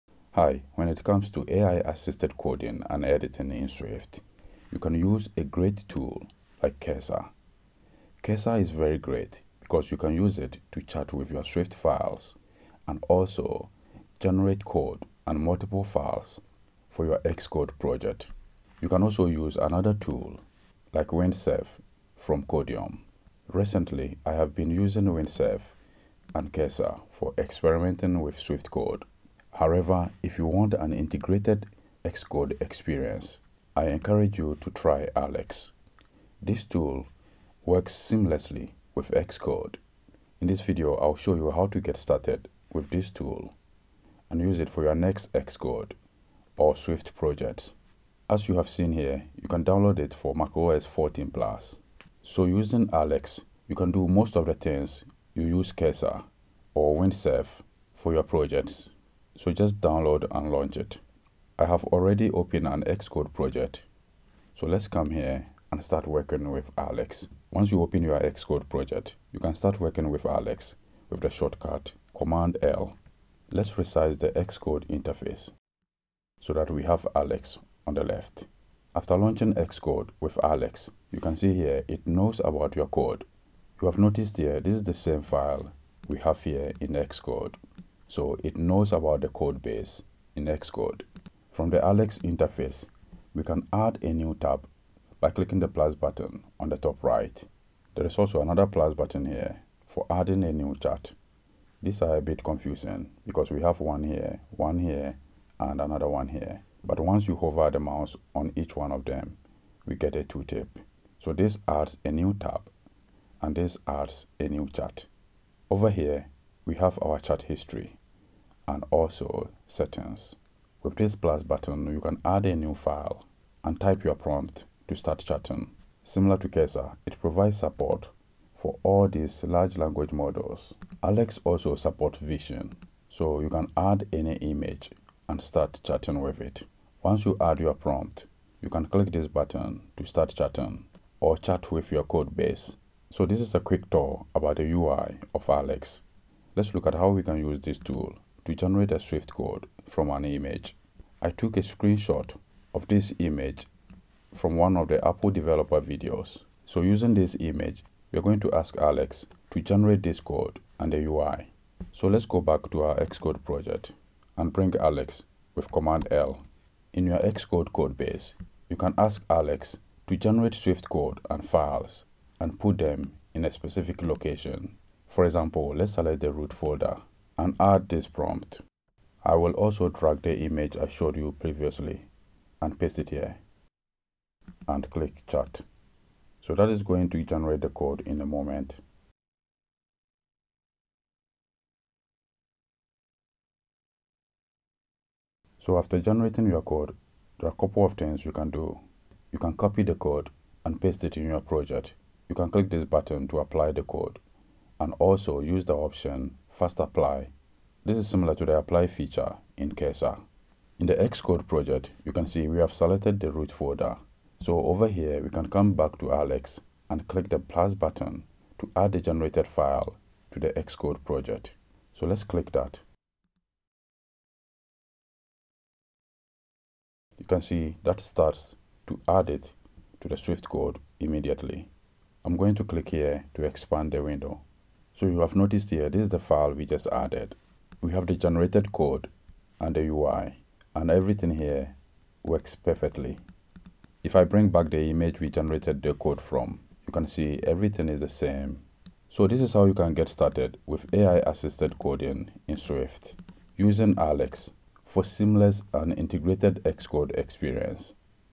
Xcode & Swift/SwiftUI AI-Assisted Coding and Editing
This video shows you how to get started with Alex, an AI code editor that offers a developer experience similar to Cursor and Windsurf.